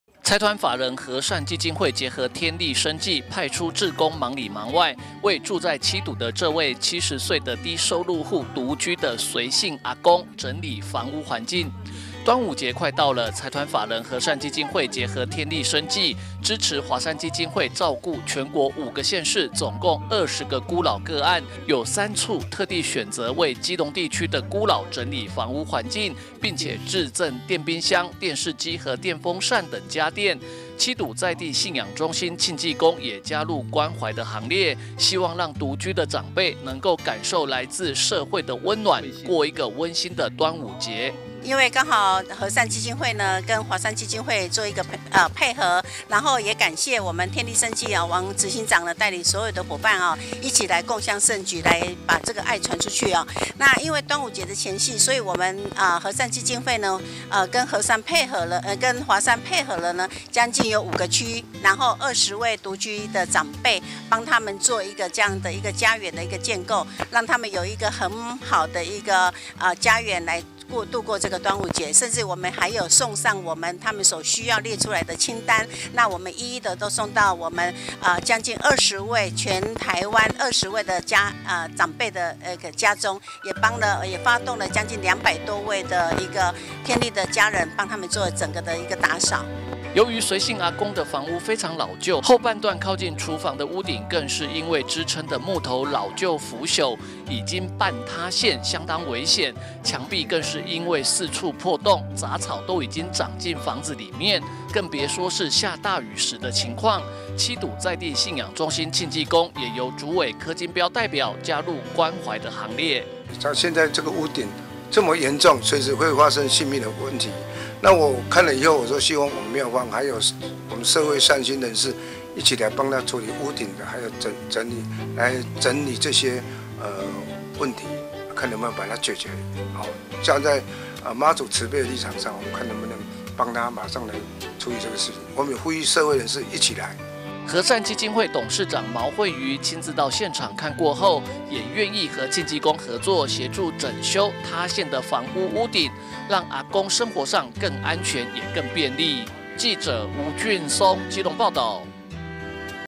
基隆|柯金標|禾繕基金會|天麗生技|華山基金會|七堵慶濟宮|七堵媽|七堵|暖暖|孤老|獨居老人|端午節|家電|整修屋頂|禾繕基金會結合天麗生技 助華山孤老過端午
禾繕基金會結合天麗生技 助華山孤老過端午

(中嘉吉隆新聞記者吳俊松/基隆報導)端午節快到了，財團法人禾繕基金會結合天麗生技，支持華山基金會照顧孤老，有三處特地選擇為基隆地區的孤老整理房屋環境，並且致贈電冰箱、電視機和電風扇等家電，七堵在地信仰中心慶濟宮也加入關懷的行列，希望讓獨居的長輩，能夠感受來自社會的溫暖，過一個溫馨的端午節！

財團法人禾繕基金會結合天麗生技，派出志工忙裡忙外，為住在七堵的這位70歲的低收入戶獨居的隨姓阿公整理房屋環境。

端午節快到了，財團法人禾繕基金會結合天麗生技，支持華山基金會照顧全國五個縣市總共20個孤老個案，有三處特地選擇為基隆地區的孤老整理房屋環境，並且致贈電冰箱、電視機和電風扇等家電，七堵在地信仰中心慶濟宮也加入關懷的行列，希望讓獨居的長輩，能夠感受來自社會的溫暖，過一個溫馨的端午節！

由於阿公的房屋非常老舊，後半段靠近廚房的屋頂更是因為支撐的木頭老舊腐朽，已經半塌陷相當危險危險，牆壁更是因為四處破洞，雜草都已經長進房子裡面，更別說下大雨時的情況，七堵在地信仰中心慶濟宮也由主委柯金標代表加入關懷的行列！

禾繕基金會董事長毛卉瑜親自到現場看過後，也願意和慶濟宮合作，協助整修坍陷的房屋屋頂，讓阿公生活上更安全便利！